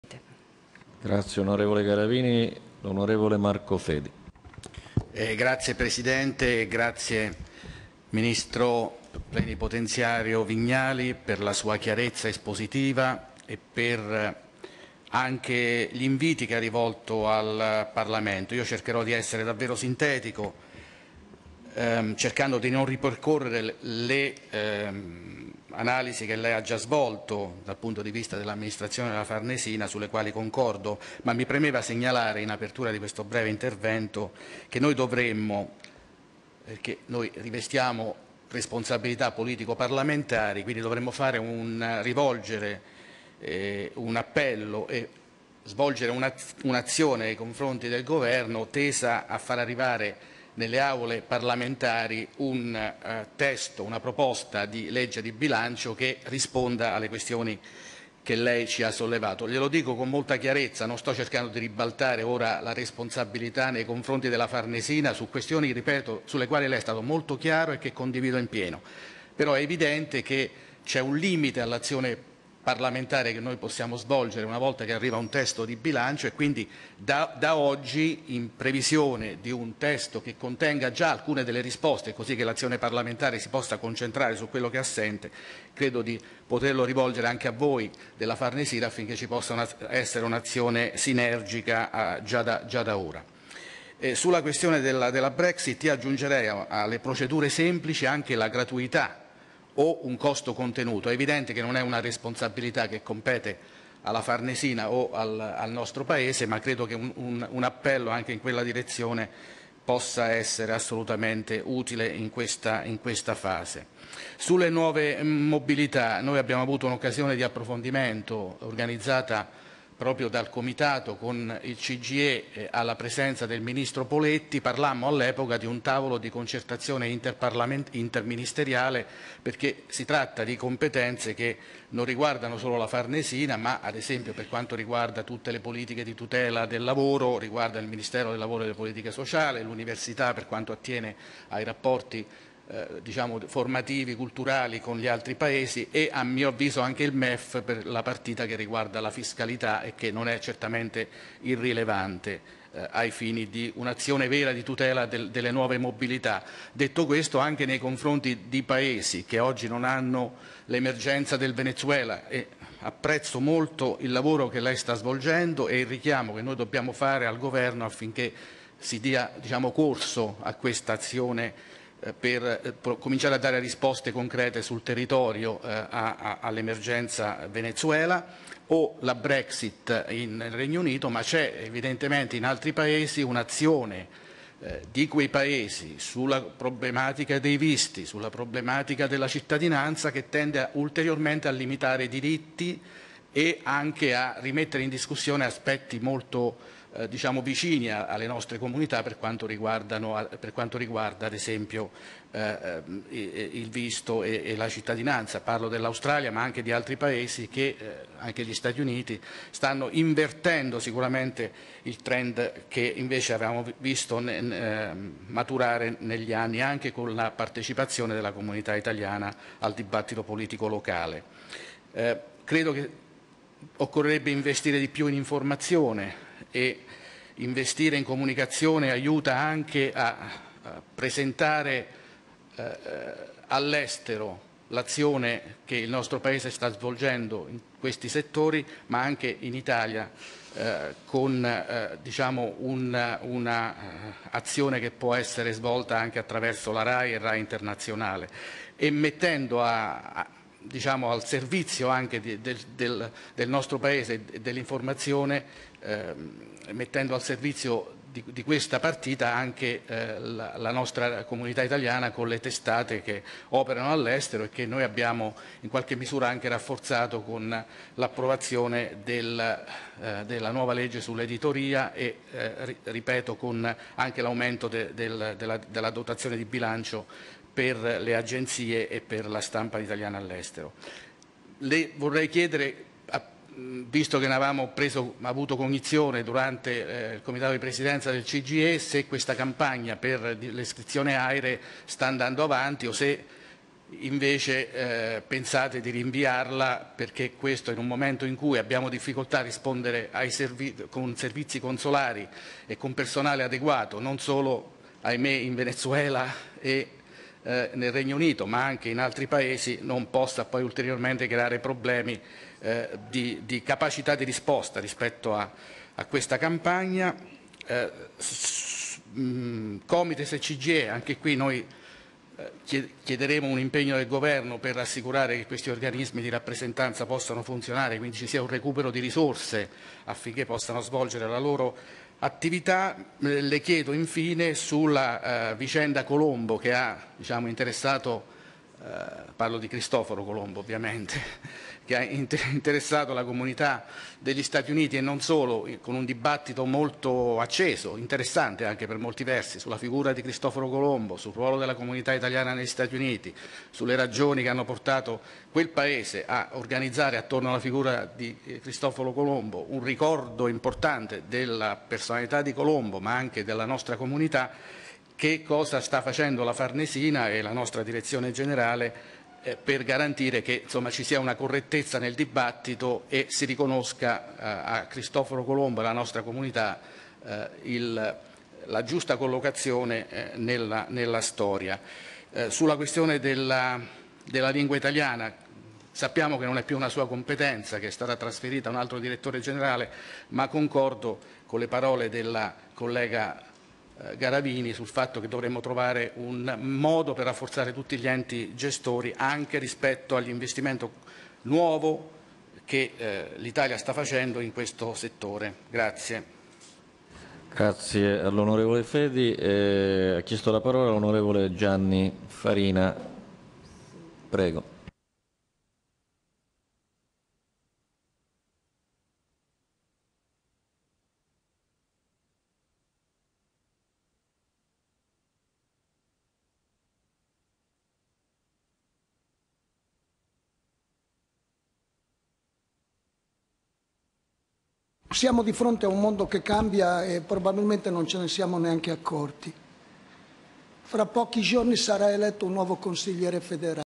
AUDIZIONE DEL DIRETTORE VIGNALI: L'INTERVENTO DELL'ON. MARCO FEDI
Giovedì 14 Settembre 2017 ore 09:00
COMITATO PERMANENTE ITALIANI NEL MONDO - Audizione Vignali, direttore italiani all'estero
Comitato permanente sugli Italiani nel mondo e la promozione del sistema paese, presso l'Aula della Commissione Esteri, ha svolto l'audizione del direttore generale per gli italiani all’estero e le politiche migratorie del Maeci, Ministro Plenipotenziario, Luigi Maria Vignali.